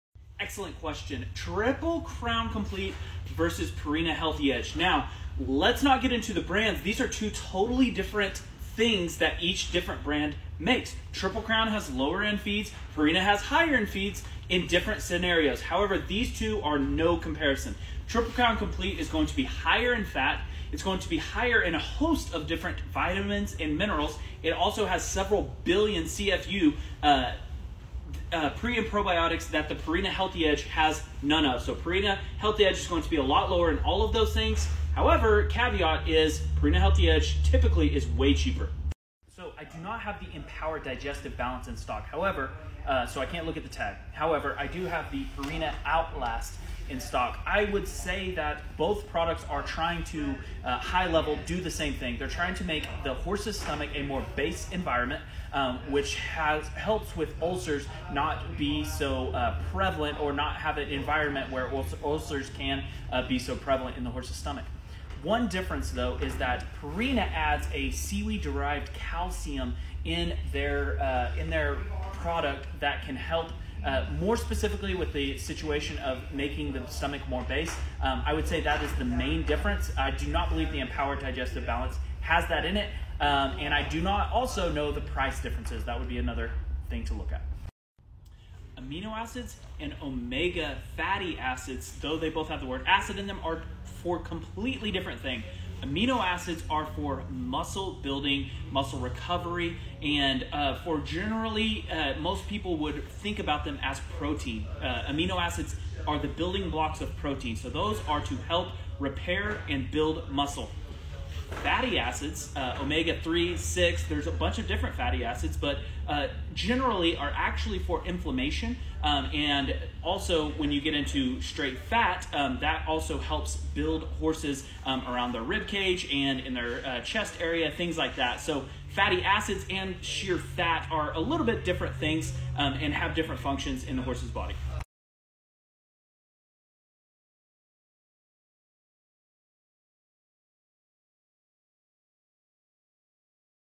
Comparing Products for Your Horses
In this episode of Steve's Horse Show, we are answering some of your questions and comparing products for your horses!

If you're ever curious about how two different products stack up against each other, watch this video! I compare a few popular horse products and answer some questions about which one might be better for your situation.

See more of what we're up to in the central Oklahoma horse industry